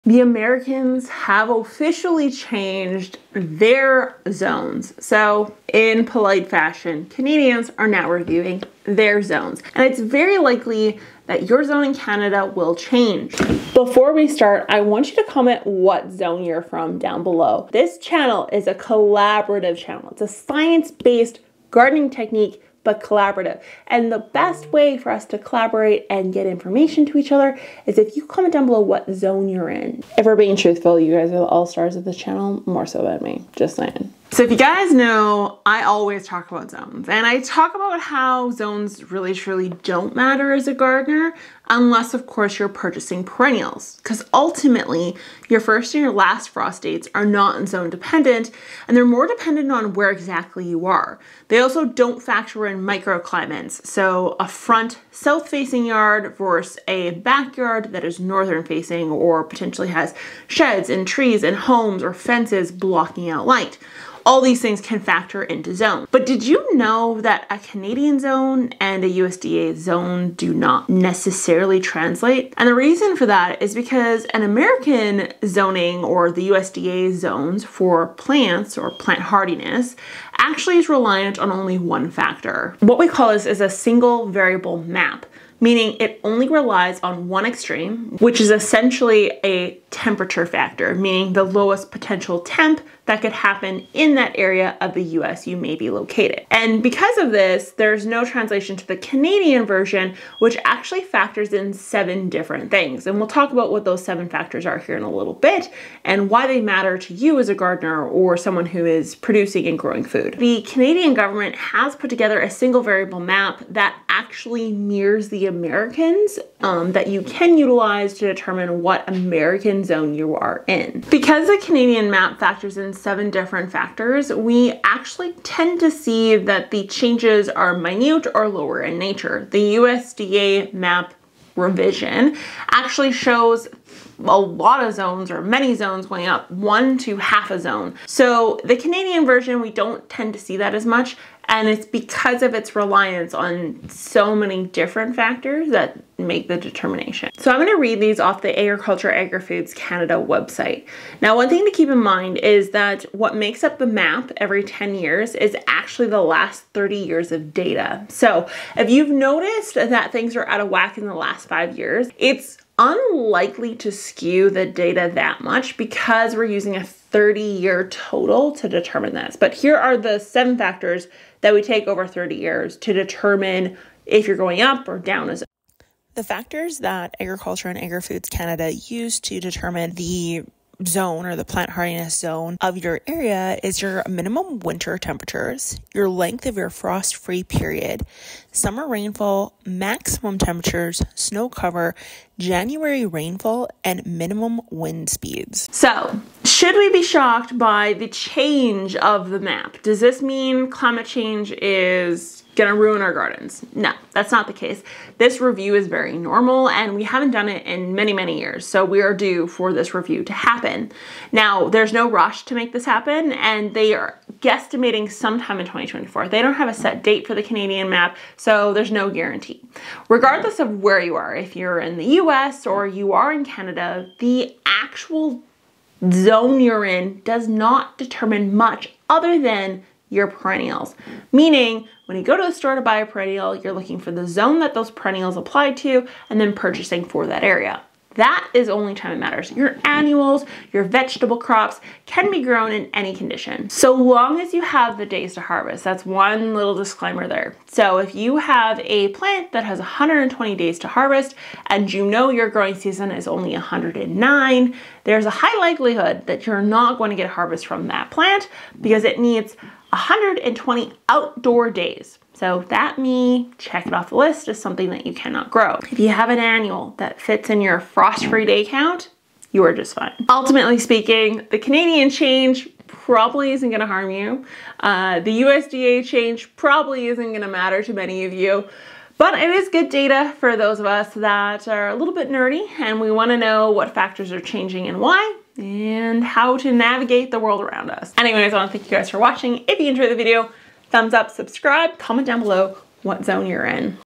Canadian Garden Zones Are Changing!?
Canada is reviewing its plant hardiness zones and there is likely going to be a small change. We will look at the Canadian map that matches the US garden zone map.

Paperback Houseplant Planner:
Paperback Garden Planner:
Digital Download Planners

Join our monthly newsletter! newsletter.gardeningincanada.net